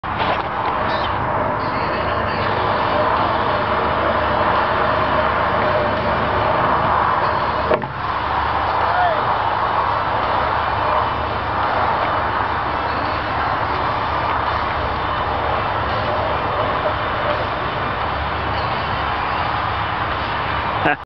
swinging is fun
first time swinging my friend Dan